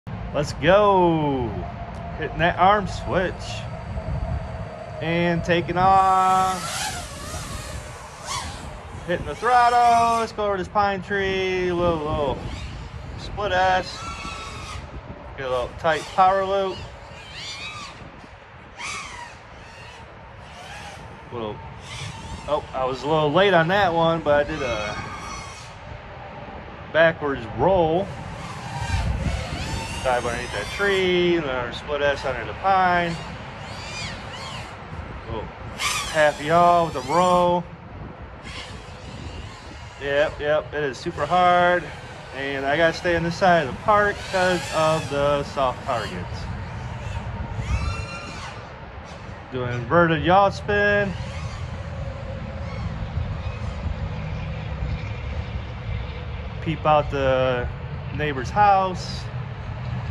BotGrinder Botcoin challenge Snz 3 Narrating while you fly
FPV Equipment:
Frame: BrockLeeFPV PLAYER 1
Vtx: DJI Caddx Vista Nebula Pro cam and Air Uint
Radio Transmitter: Taranis X9D 2019 Se
Radio Link: TBS CrossFire Micro TX
Radio Reciever: TBS CrossFire Nano RX
Motor: NewBeeDrone Flow Motors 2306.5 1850KV